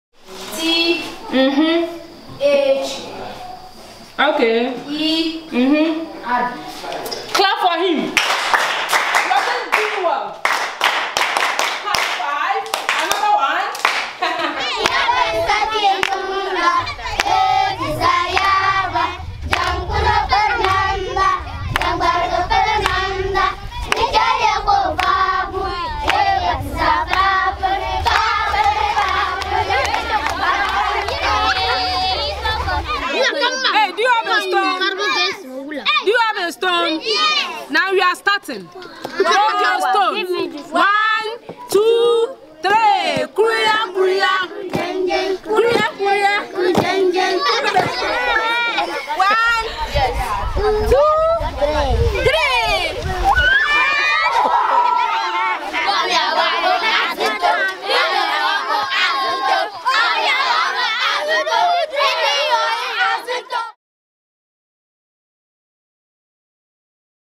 Educo - Aprender jugando en Ghana
Conoce un poco más uno de los proyectos que llevamos a cabo en 40 escuelas de la región norte de Ghana junto a la ONG local PAORP-Panafrican. La educación mediante el juego.